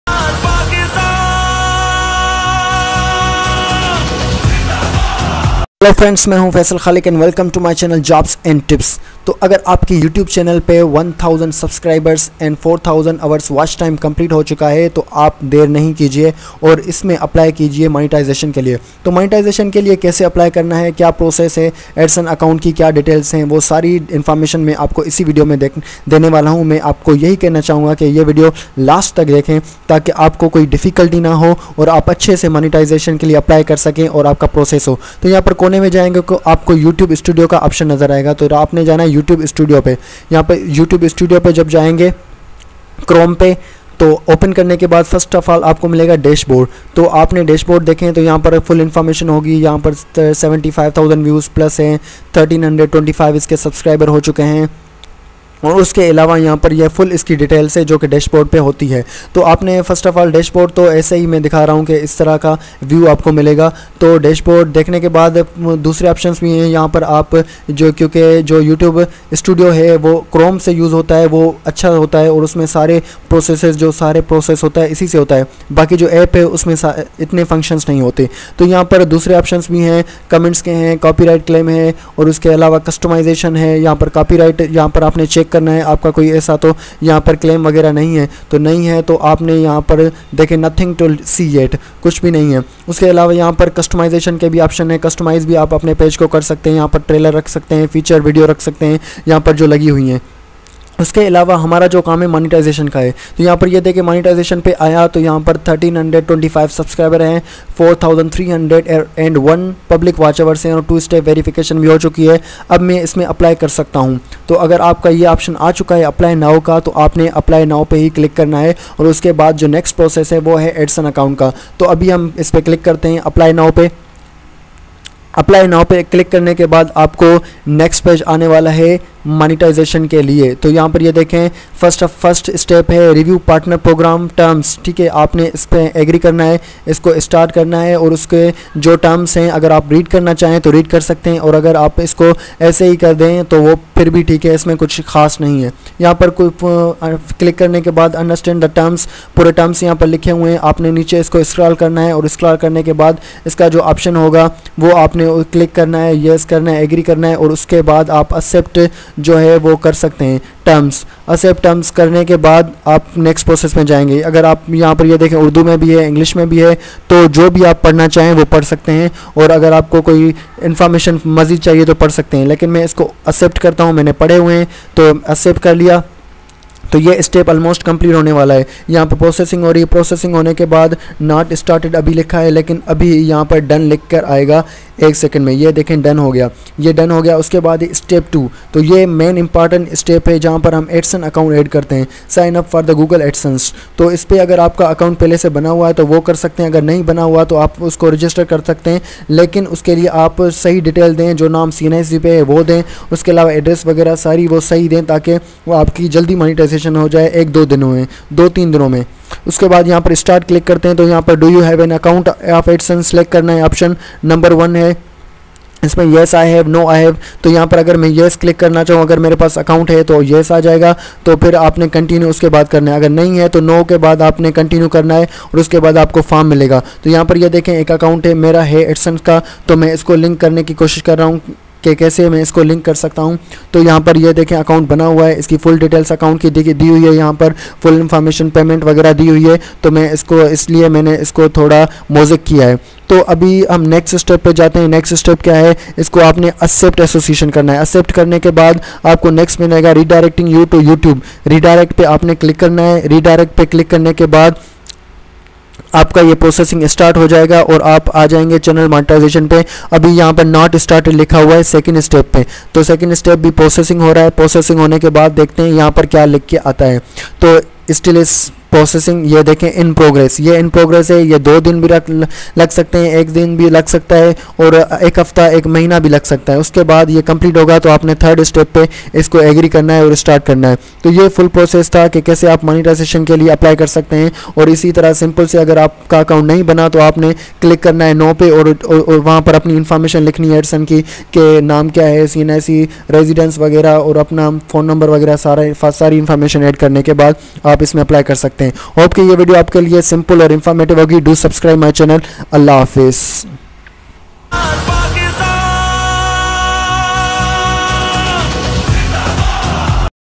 How to monetize YouTube channel in 2022 | How to link adsense account with YouTube
This video contain full information about
How to apply  for monetization
How to link adsence account
How to agree YouTube  partner program
How to create  adsense account